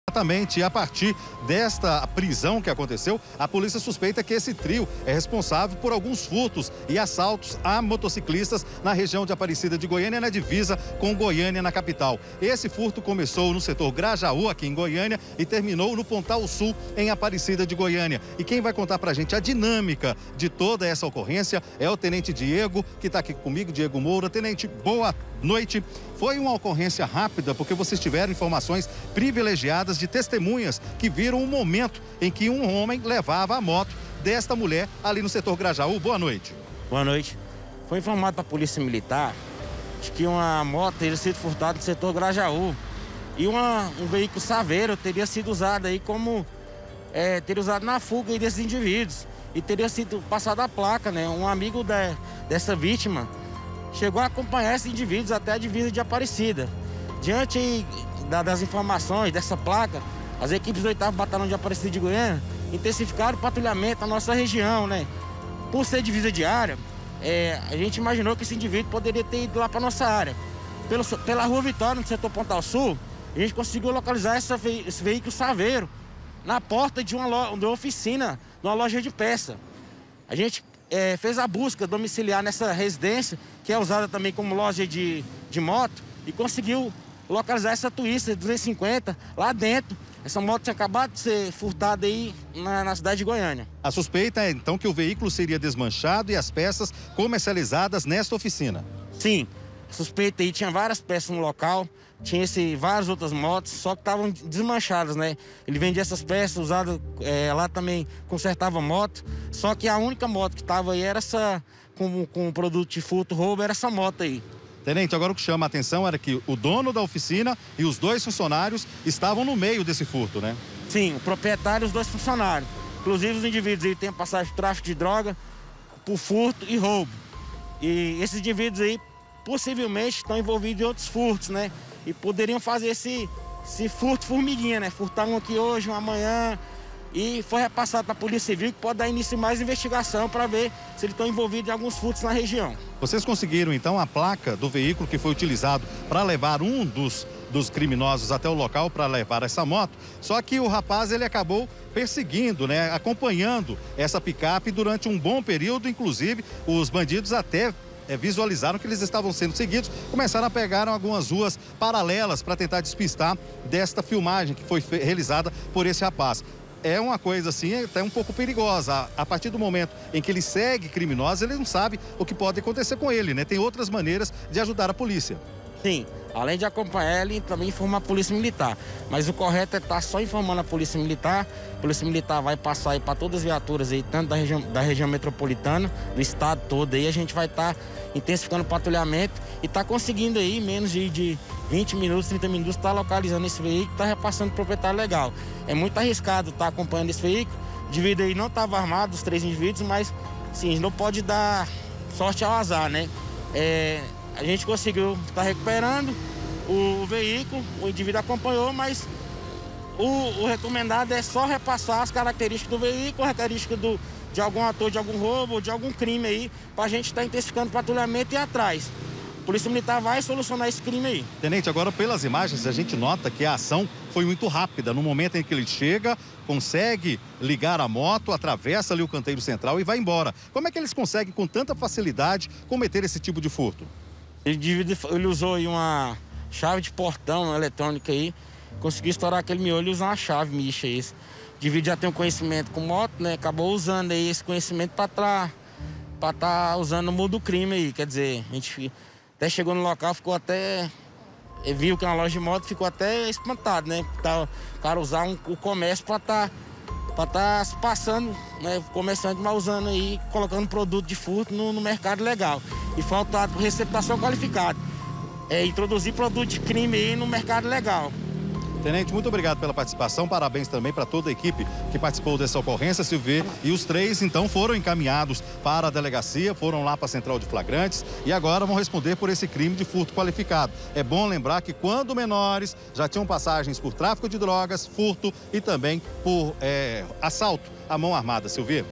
O ATAQUE DO TRIO: EMPRESÁRIO É PRESO SUSPEITO DE FURTAR MOTO
TRÊS HOMENS FORAM PRESOS SUSPEITOS DE FURTAR MOTOS EM APARECIDA. O QUE CHAMA A ATENÇÃO É QUE UM DELES É DONO DE UMA LOJA DE MOTOS.